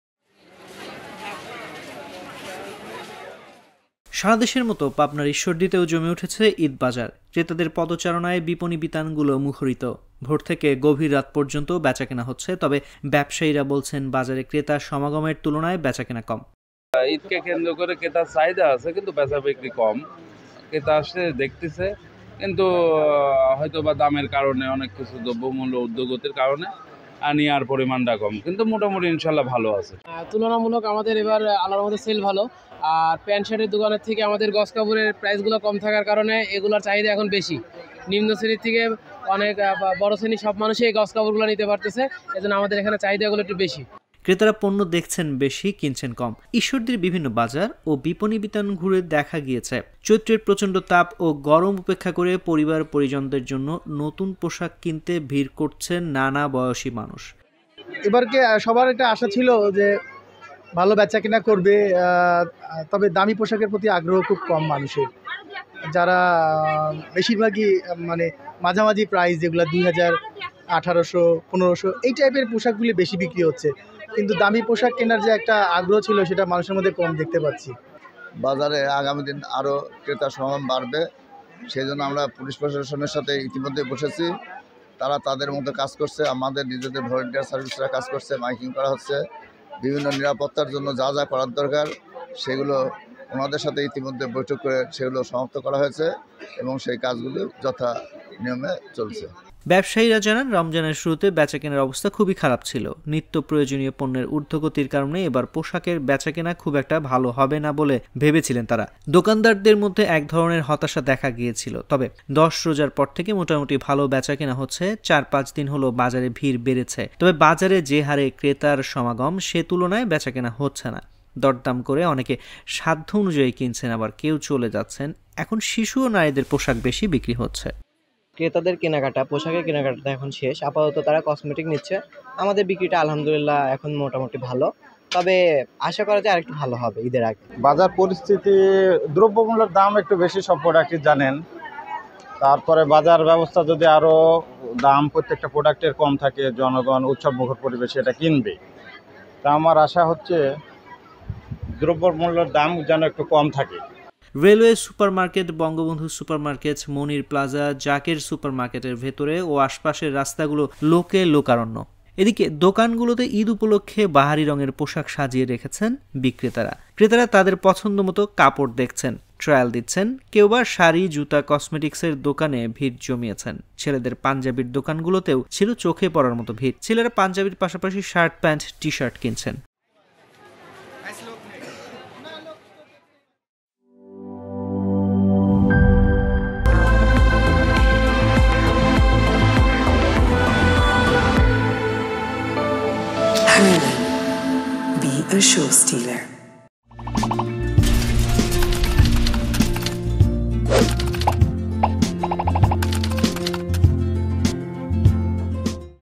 বাজারে যে হারে ক্রেতার সমাগম সে তুলনায় বেচাকেনা হচ্ছে না | Jago News
বাজারে যে হারে ক্রেতার সমাগম সে তুলনায় বেচাকেনা হচ্ছে না

সারাদেশের মতো পাবনার ঈশ্বরদীতেও জমে উঠেছে ঈদ বাজার। ক্রেতাদের পদচারণায় বিপণিবিতানগুলো মুখরিত। ভোর থেকে গভীর রাত পর্যন্ত বেচাকেনা হচ্ছে। তবে ব্যবসায়ীরা বলছেন, বাজারে ক্রেতা সমাগমের তুলনায় বেচাকেনা কম। ক্রেতারা পণ্য দেখছেন বেশি কিনছেন কম।

ঈশ্বরদীর বিভিন্ন বাজার ও বিপণিবিতান ঘুরে গিয়ে দেখা যায়, চৈত্রের প্রচন্ড তাপ ও গরম উপেক্ষা করে পরিবার-পরিজনদের জন্য নতুন পোশাক কিনতে ভিড় করছেন নানা বয়সী মানুষ। রেলওয়ে সুপার মার্কেট, বঙ্গবন্ধু সুপার মার্কেট, মনির প্লাজা, জাকের সুপার মার্কেটের ভেতরে ও আশপাশের রস্তাাগুলো লোকে লোকারণ্য।